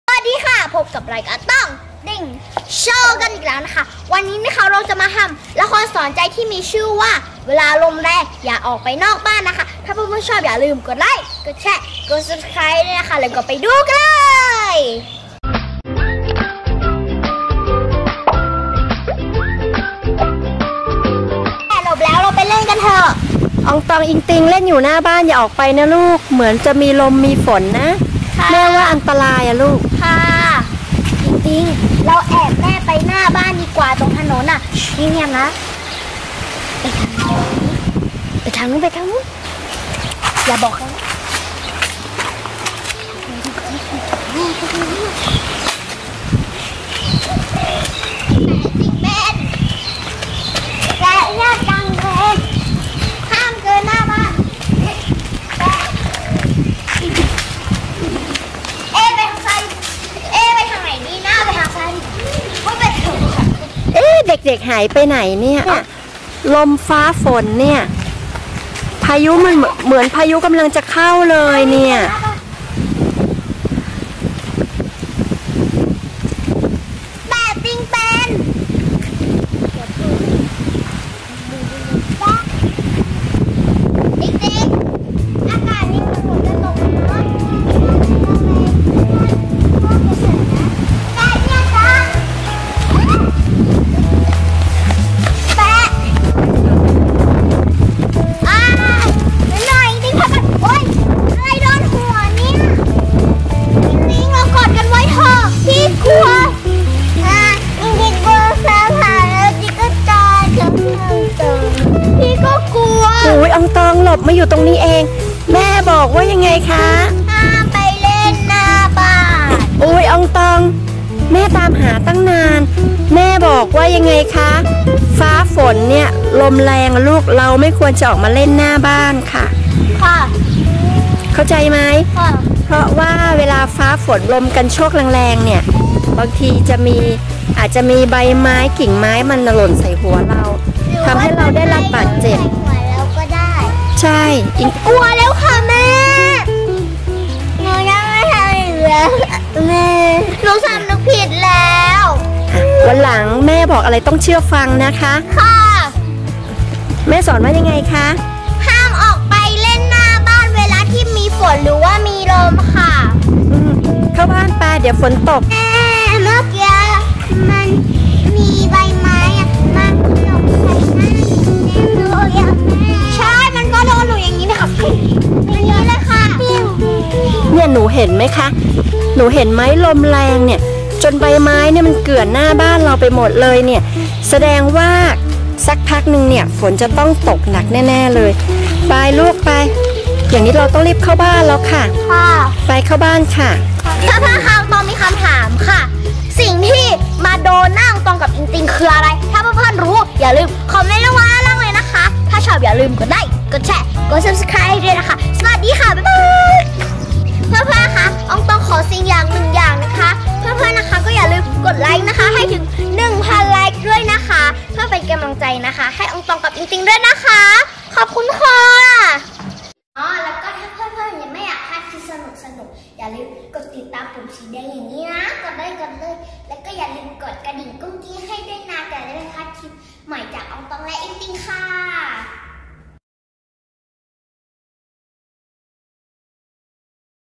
เด็กดี อย่าไปไหนเวลาฝนตก ลมแรง I ละครตอง ติง โชว์ ♥️ ตอง ติง โชว์ ♥️
5 เหตุการณ์เงิบๆ!! ที่ต้องเคยเจอในเซเว่น